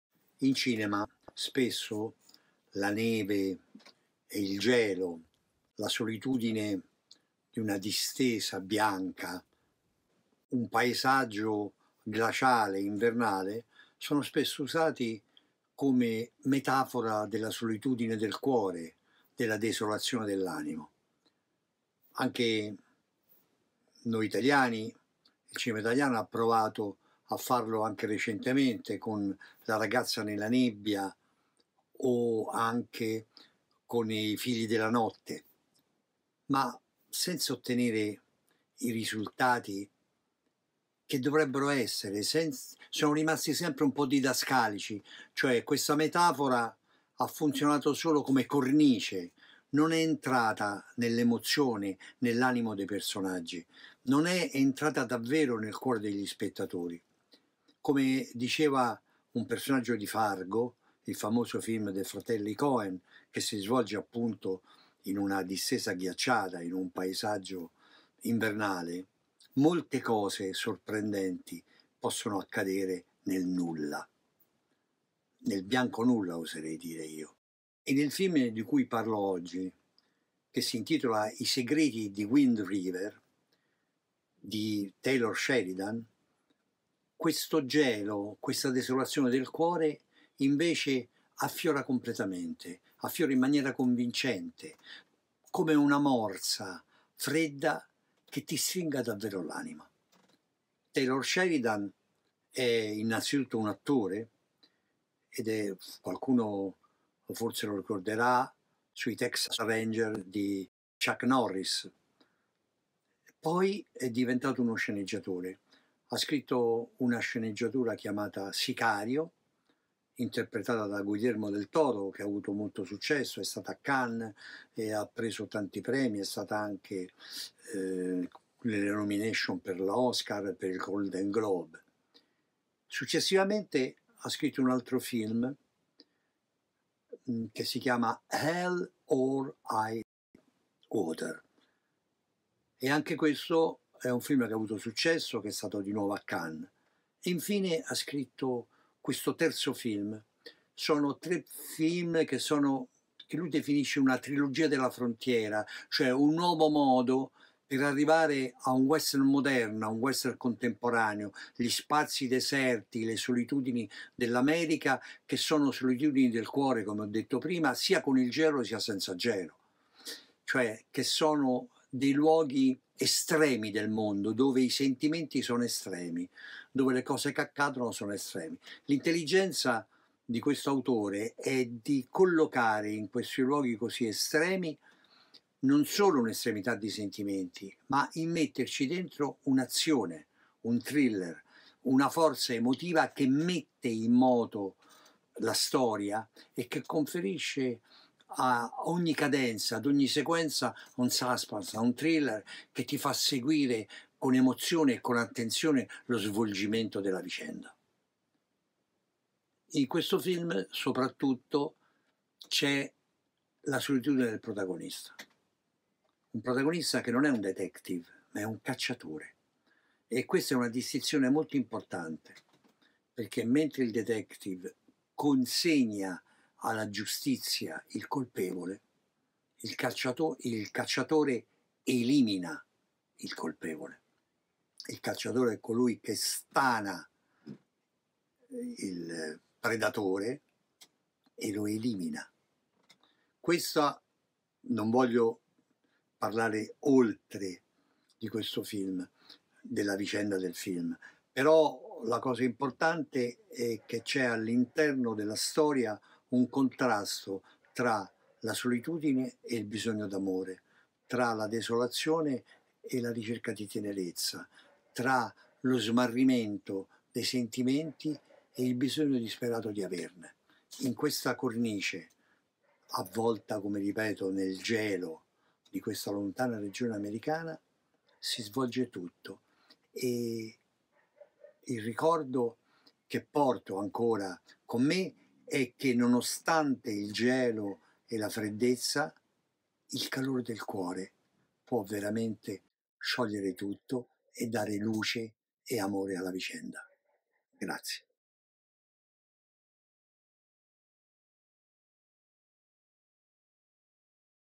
WIND RIVER - movie review by Roberto Leoni Eng sub
Roberto Leoni reviews Wind River written and directed by Taylor Sheridan starring Jeremy Renner Elizabeth Olsen